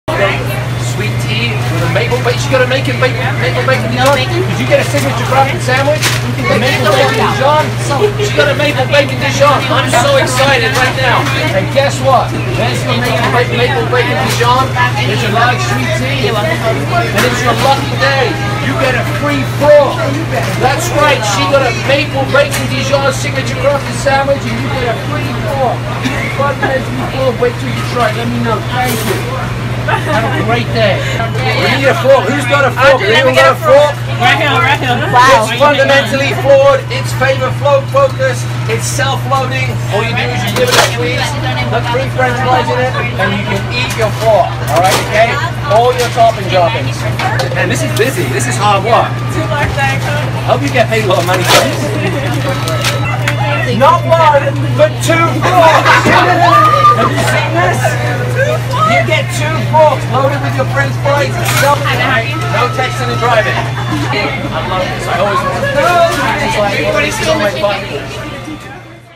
'Pitchman' Anthony Sullivan works a McDonald's drive-thru
Anthony Sullivan of infomercial fame hands out 'frorks' as he works the drive-thru at a Tampa McDonald's.